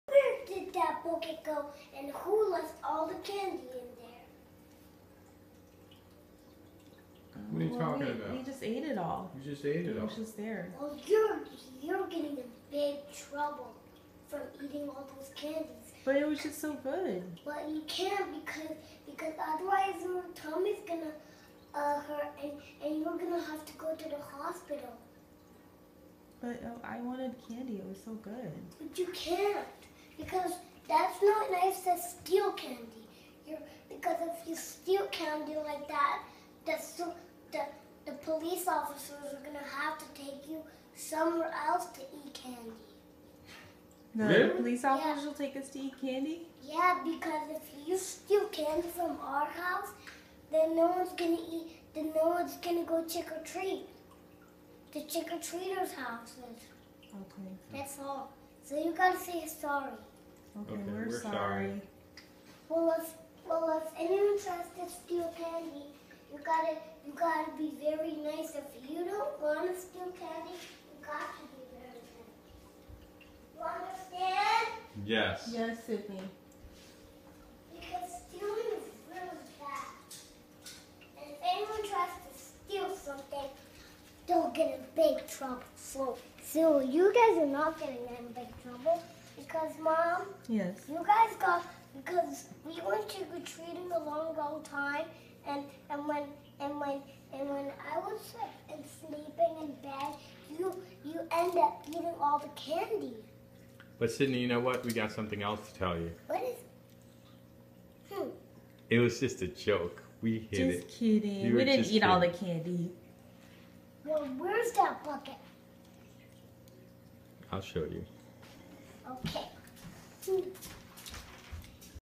Hey Jimmy Kimmel, I Told My Kids I Ate Their Halloween Candy 2015
Sydney finds out Mommy and Daddy got into his candy!